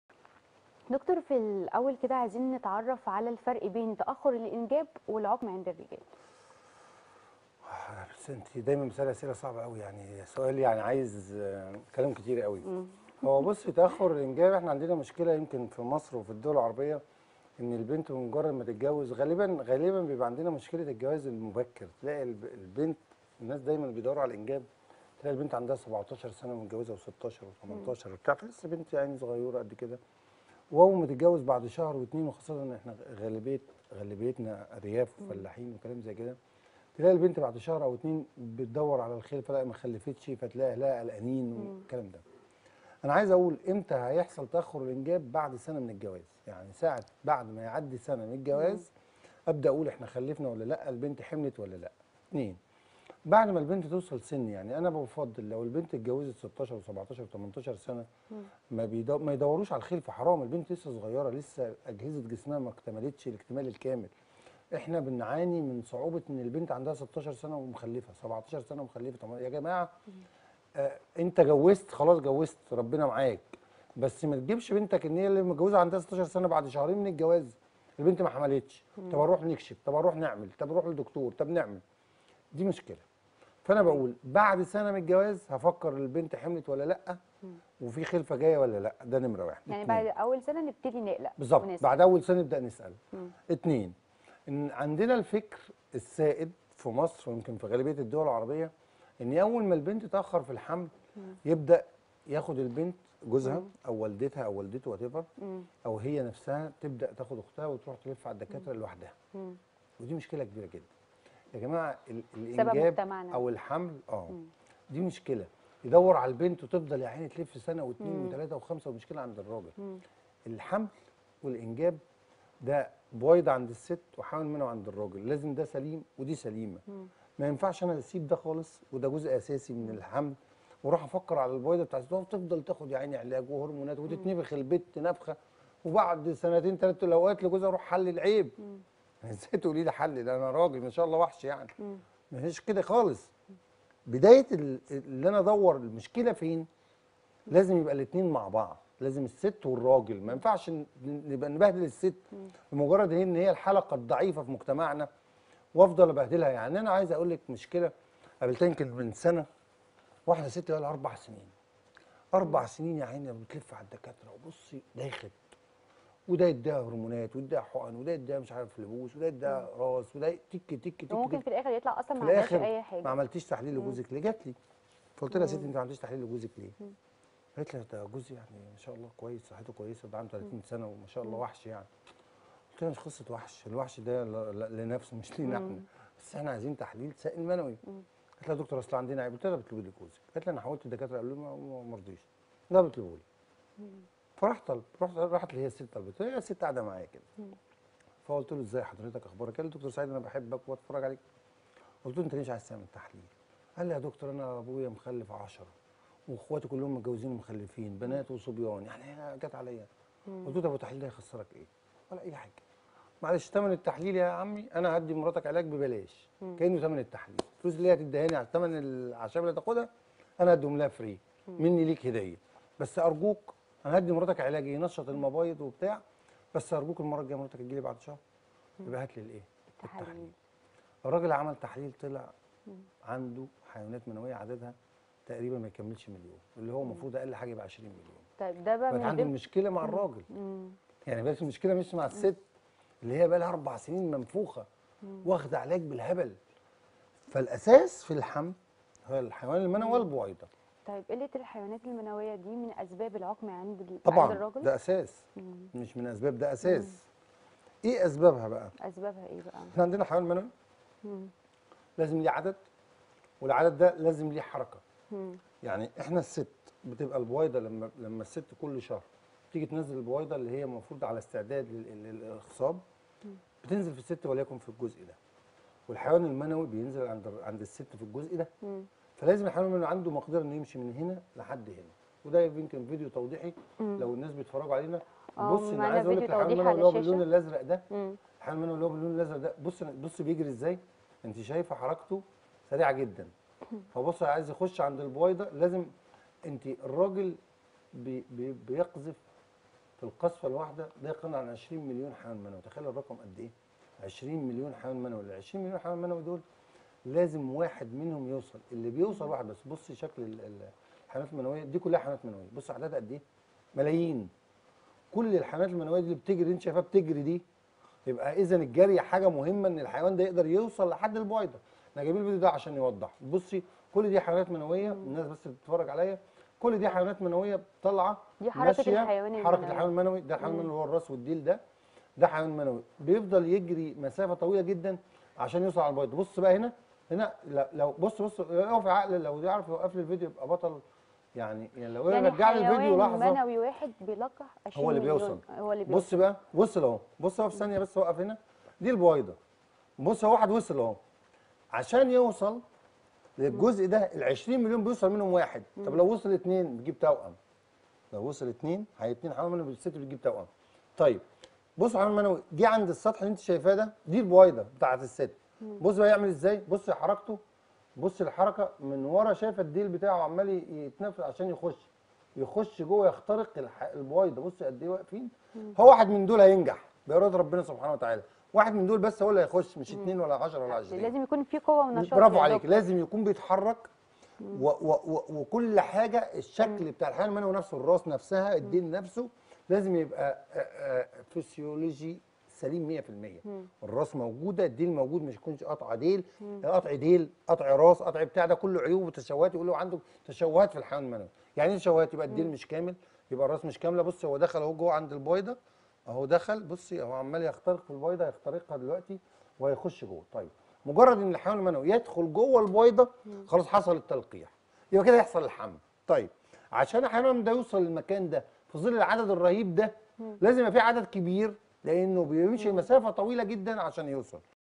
ما هو الفرق بين تأخر الإنجاب وبين والعقم عند الرجال مع دكتور سعيد حساسين
تعرف على الفرق بين تأخر الأنجاب والعقم عند الرجال مع دكتور سعيد حساسين

قناة_الصحة_والجمال# برامجنا